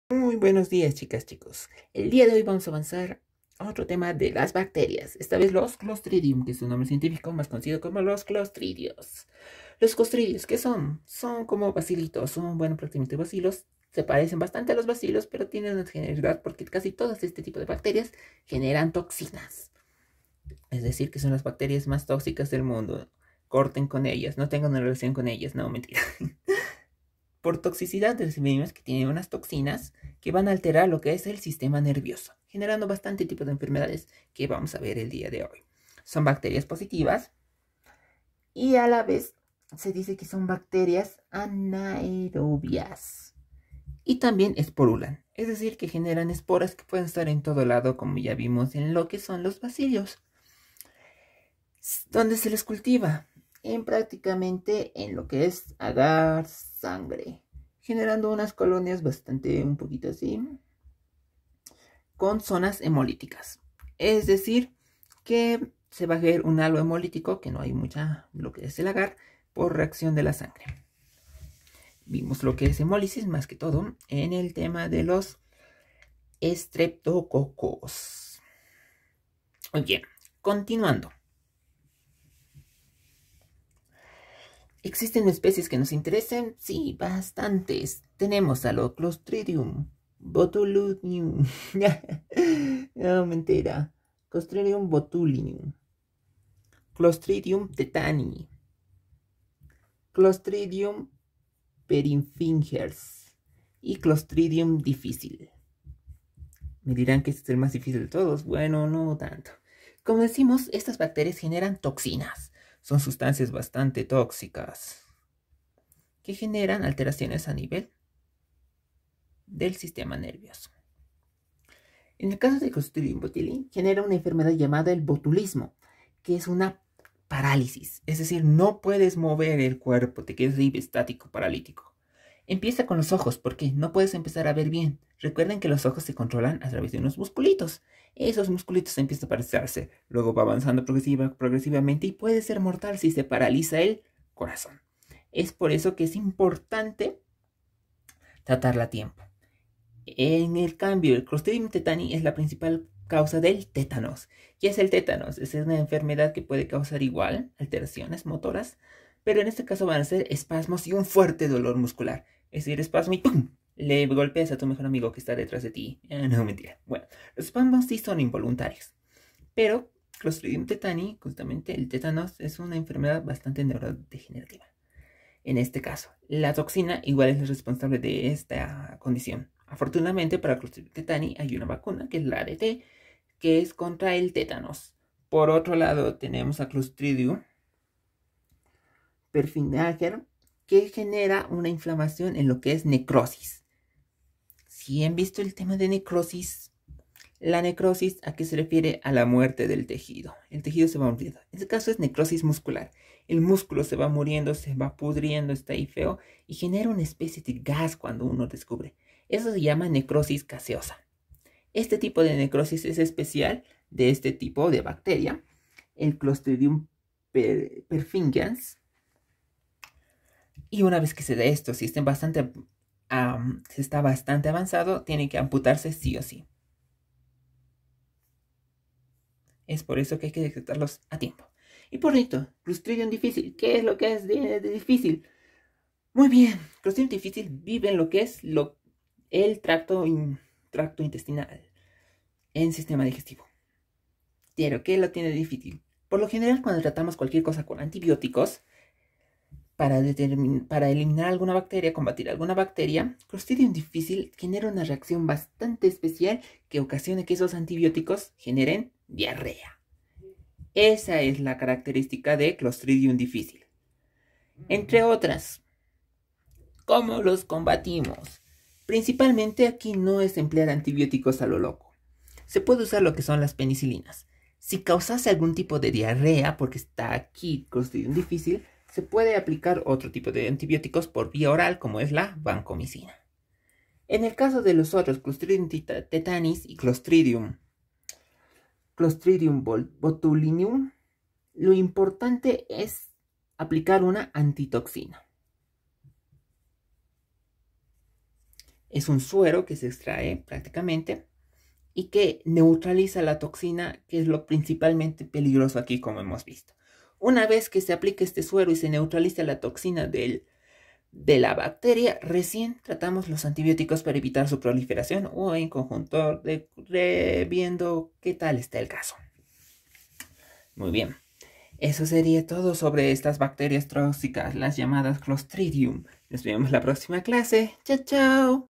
Clostridios
Bacterias que secretan toxinas. Veamos a los clostridios.